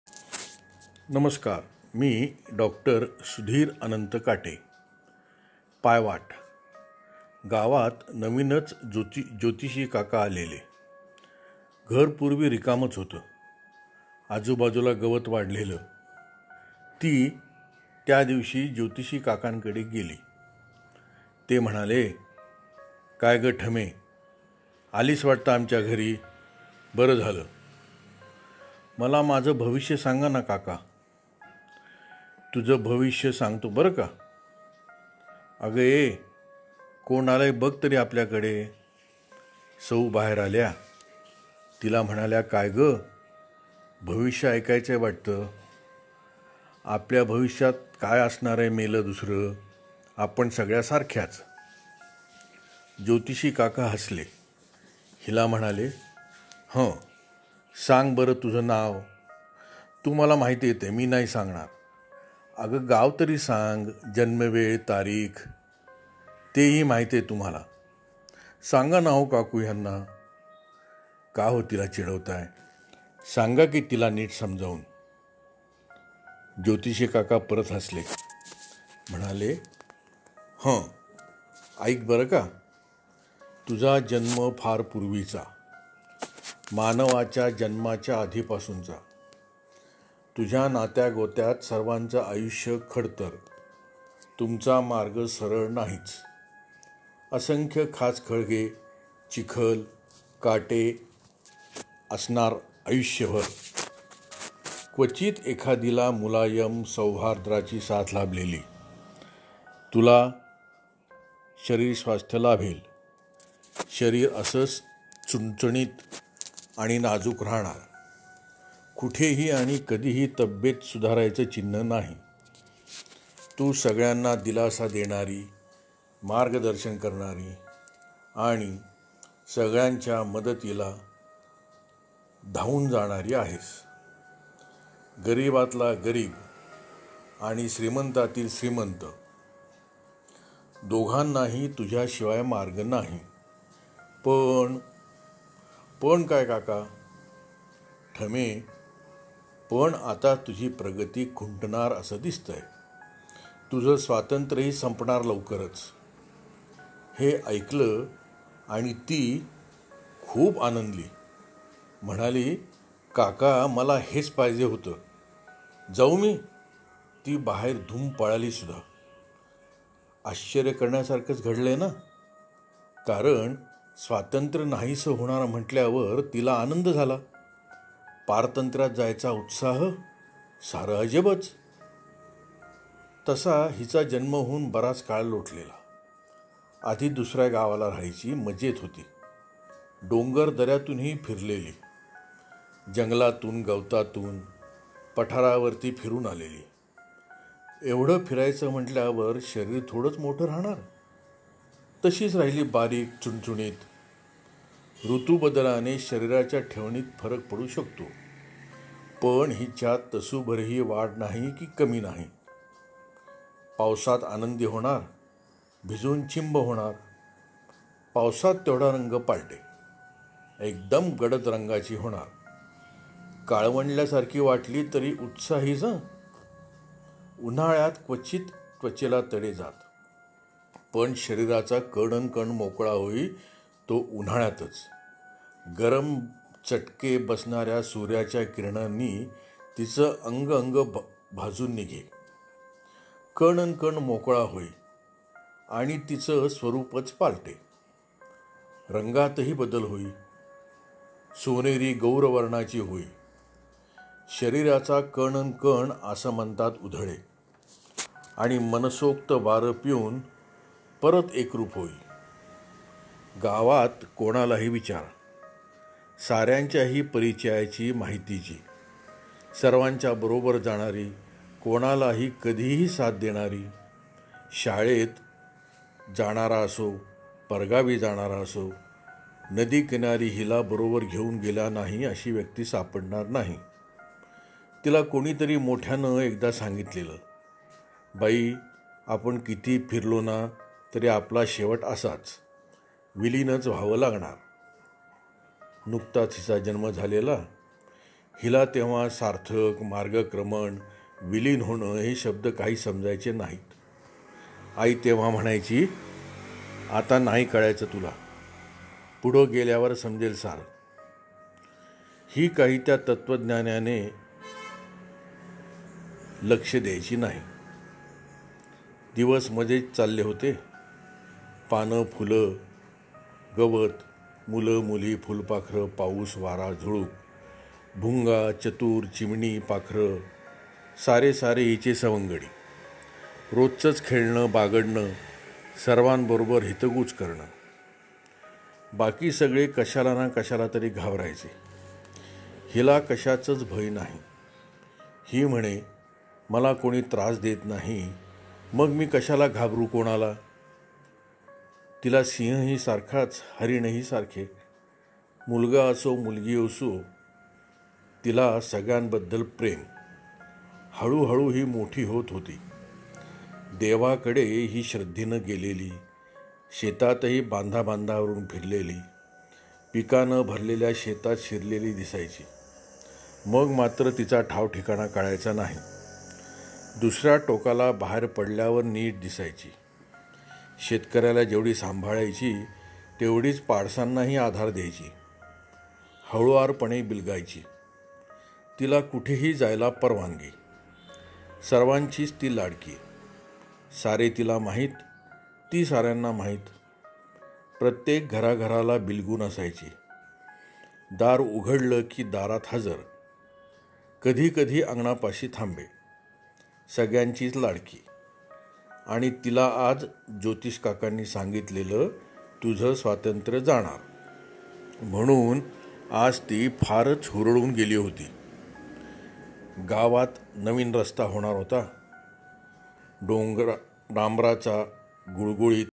by sudhir Kate
It's a PATHWAYstory and short poem,charoli,describing nature